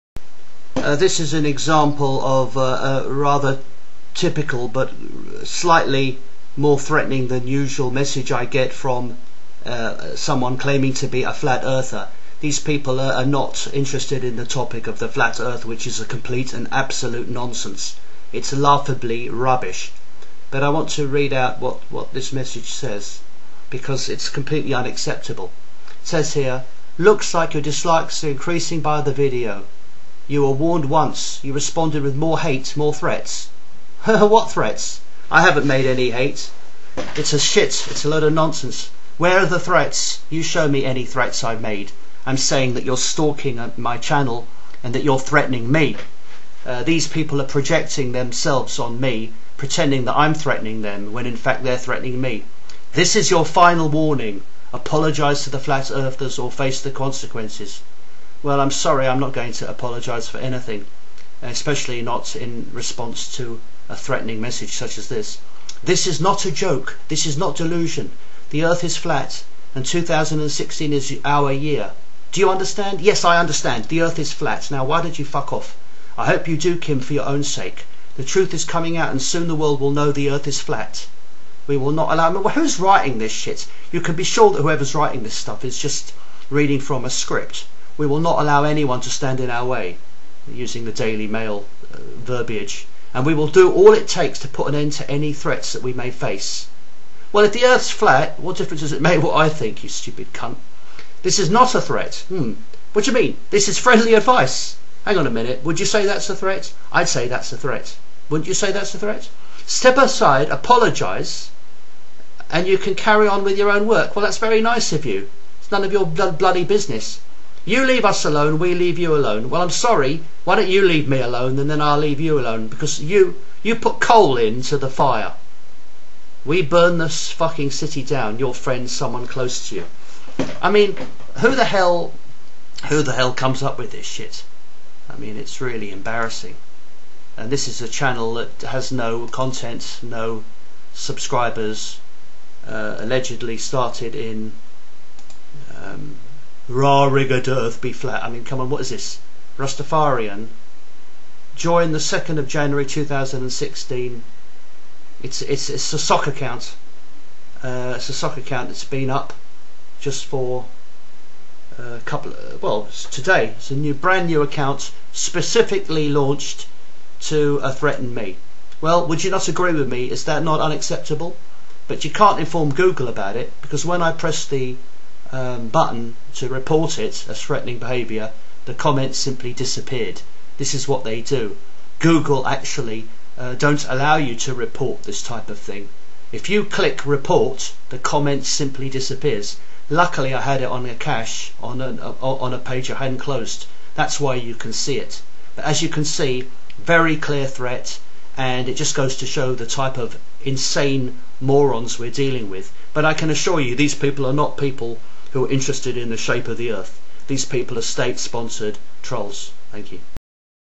Get a new threat from a flat earther
Rah Digga Da Earth Be Flat 13 minutes ago · Shared privately
Looks like your dislikes are increasing by the video!
You were warned once, you responded with more hate, more
threats. This is your final warning. Apologise to the Flat Earther's,
or face the consequences.
This is not a joke, this is not delusion. The Earth is Flat and 2016
is OUR year. Do you understand? I hope you do Kim, for your
own sake.The truth is coming out and soon the world will know
the Earth is Flat. We will not allow ANYONE to stand in our way.
And we will do all it takes to put an end to any threats that we
may face.
threat channels
This is not a threat, its friendly advice. Step aside, apologise and
you can carry on with your own work. You leave us alone, we
leave you alone. You put coal in to the fire, we burn the fucking
city down.
Your friend,
Someone close to you.